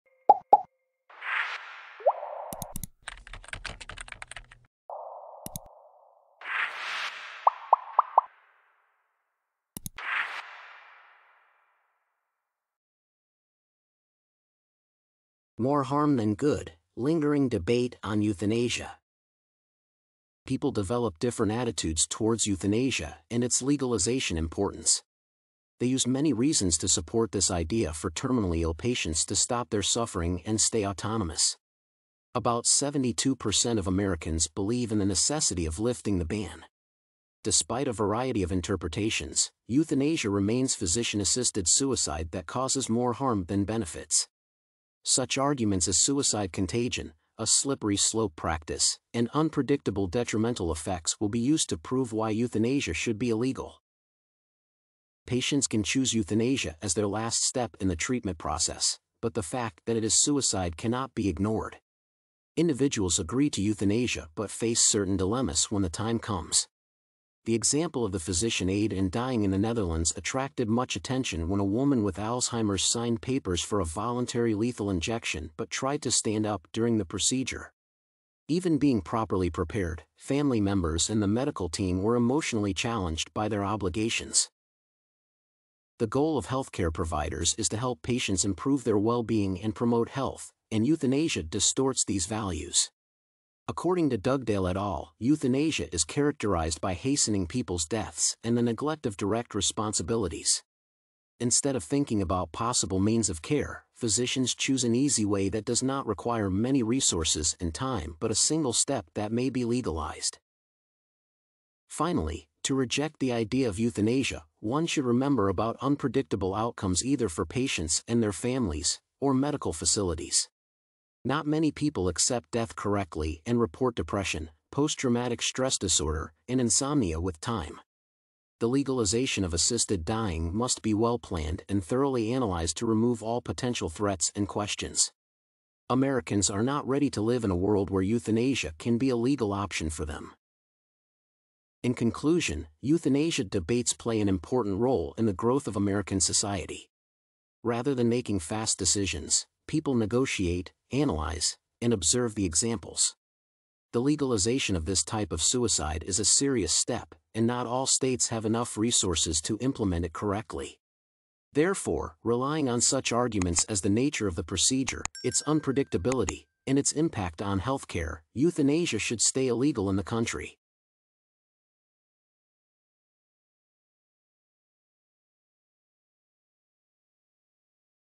More Harm Than Good: Lingering Debate on Euthanasia - Essay Example
📝 Essay description: Despite a variety of interpretations, euthanasia remains physician-assisted suicide that causes more harm than benefits. Such arguments as suicide contagion, a slippery slope practice.

ℹ️ Subjects: Medical Ethics
ℹ️ Words: 389
ℹ️ Pages: 2

📚 Reference list:
Dugdale, L. S., Lerner, B. H., & Callahan, D. (2019). Pros and cons of physician aid in dying. The Yale Journal of Biology and Medicine, 92(4), 747-750.
Lane, C. (2019). Lifting the U.S. ban on euthanasia is like opening a Pandora’s box. The Washington Post. Web.